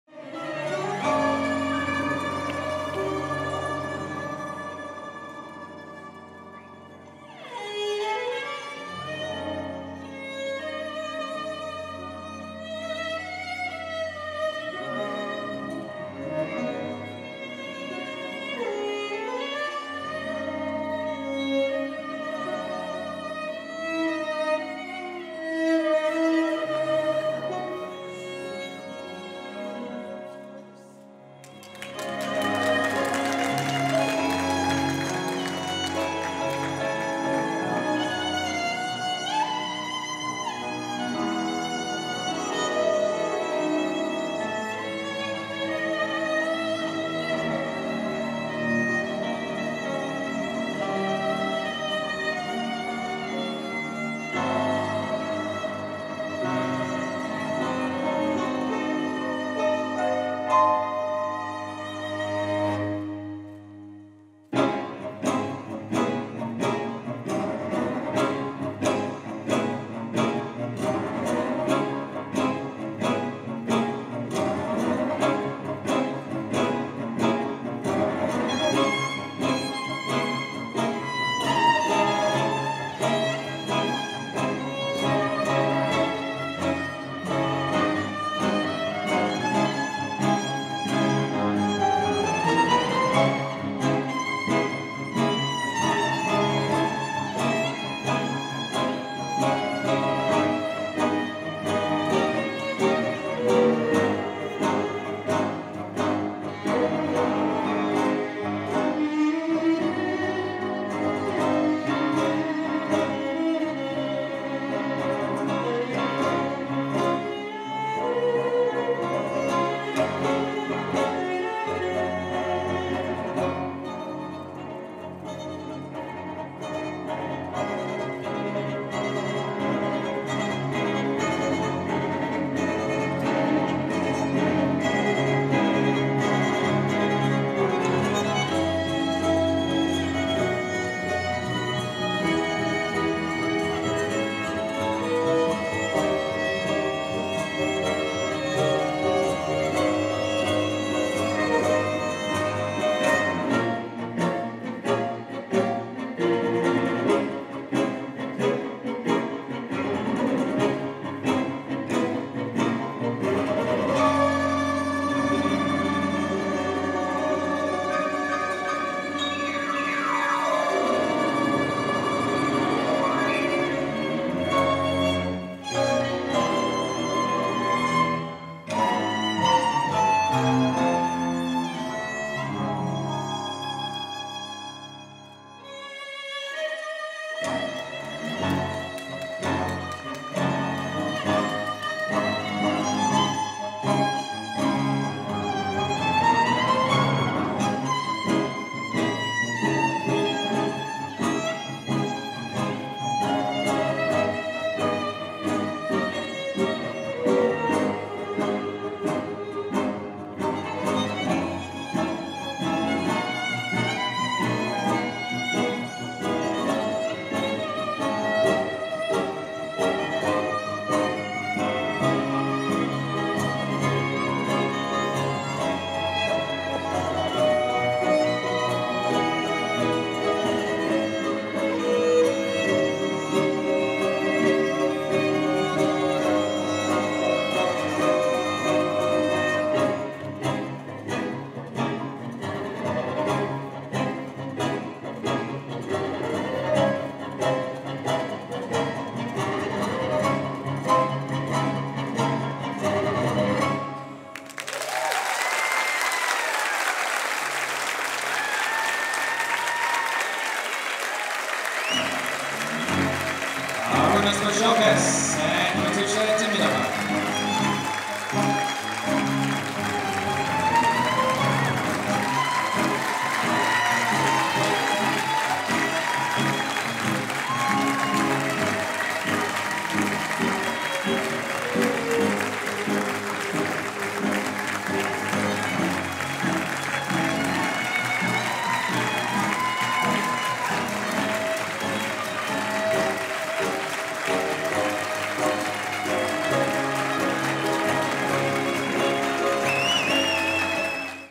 Arunas Bizokas & Katusha Demidova Tango
Arunas Bizokas & Katusha Demidova at 2013 Washington Open DanceSport Competition.